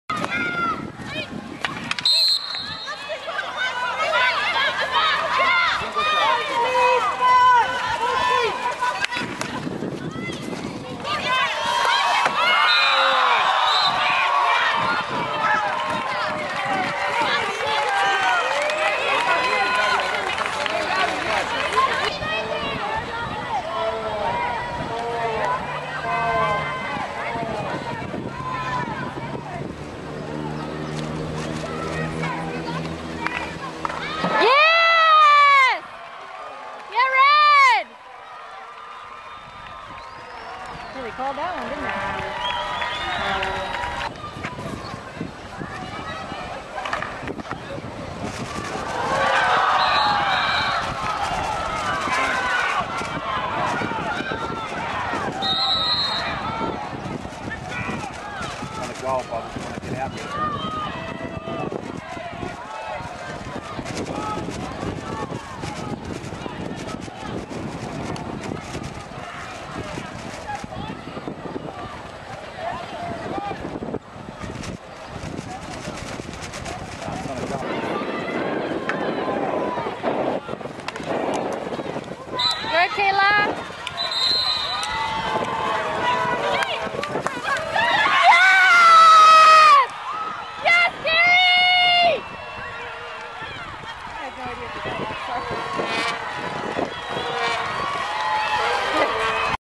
2009 Womens Pan Am Cup - USA vs. ARG - Final
USA 2 v. ARG 2 (6-7 PN)
ARG: Rebecchi, Luchetti
USA: Lingo (x2)